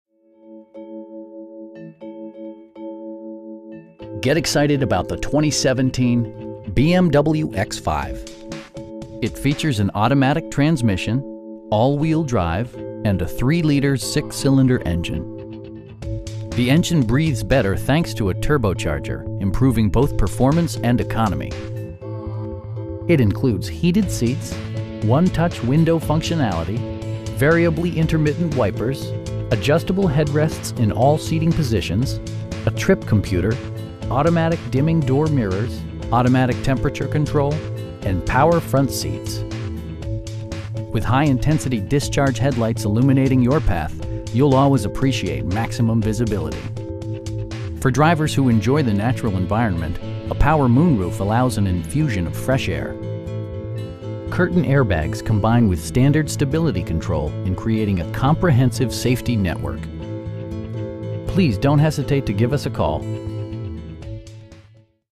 2017 BMW X5 xDrive35i in Greensboro, NC 27407
Crown BMW
3902 W. Wendover Ave.

Get excited about the 2017 BMW X5 It features an automatic transmission, all-wheel drive, and a 3 liter 6 cylinder engine. The engine breathes better thanks to a turbocharger, improving both performance and economy. It includes heated seats, 1-touch window functionality, variably intermittent wipers, adjustable headrests in all seating positions, a trip computer, automatic dimming door mirrors, automatic temperature control, and power front seats. With high intensity discharge headlights illuminating your path, you'll always appreciate maximum visibility. For drivers who enjoy the natural environment, a power moon roof allows an infusion of fresh air. Curtain airbags combine with standard stability control in creating a comprehensive safety network. We pride ourselves in the quality that we offer on all of our vehicles. Please don't hesitate to give us a call.